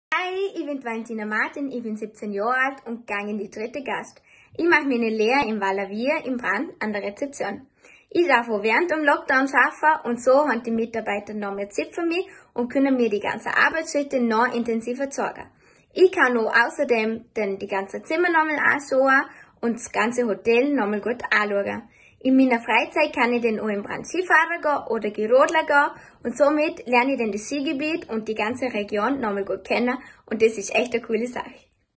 GASCHT Schuelerin Valentina, Hotel Valavier, Brand
GASCHT Schueler:innen stellen ihre Ausbildungsbetriebe vor.
Valentina, Hotel Valavier, Brand